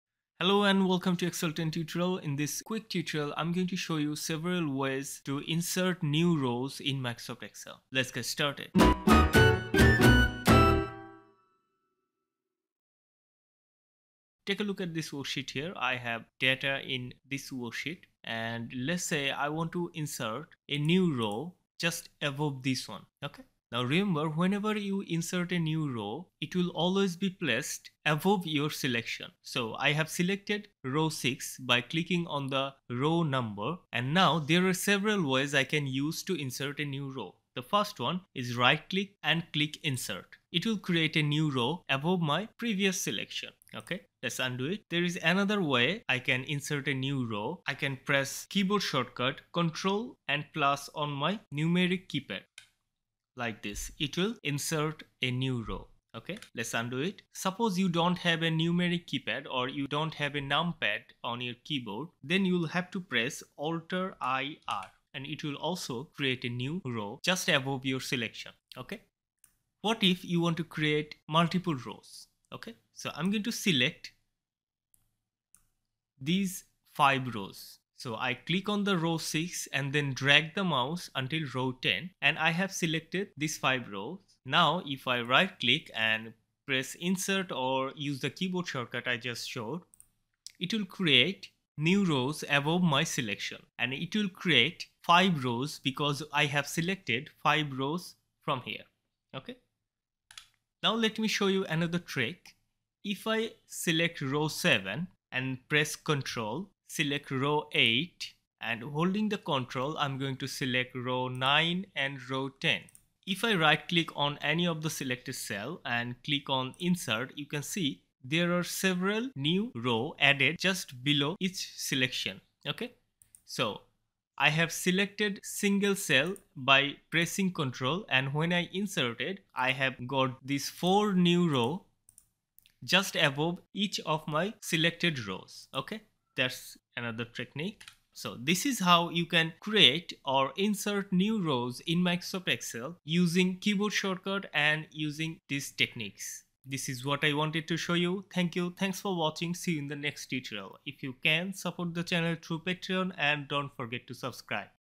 How to Insert Rows in Excel
*How to insert row/rows in excel.*
If adding a row in Excel seems challenging, don't worry – this tutorial is here to help. After watching this excel tutorial, inserting a row will be easy. Sometimes, we forget to add data or need to insert a row in a table or dataset, and Excel makes it simple. This tutorial covers the quickest and easiest way to do it. Just watch the video to learn how to insert a row in Excel.
In this tutorial you’ll learn:
How to insert a row in excel.
How to insert row using keyboard shortcuts.
How to add multiple rows in excel.
Bonus trick to insert rows in excel.